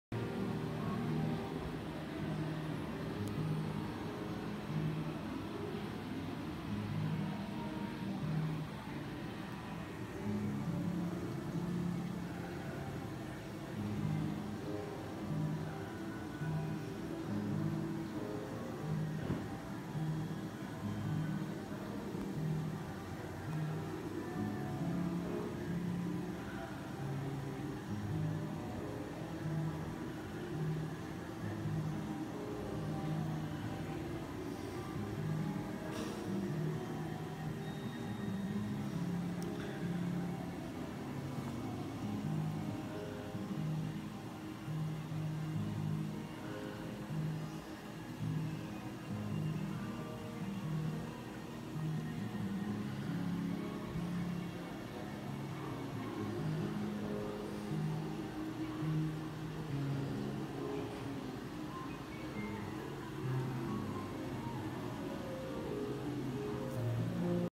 わくわく水族館のカラージェリー
イオンモール宇城で開催中のわくわく水族館のカラージェリー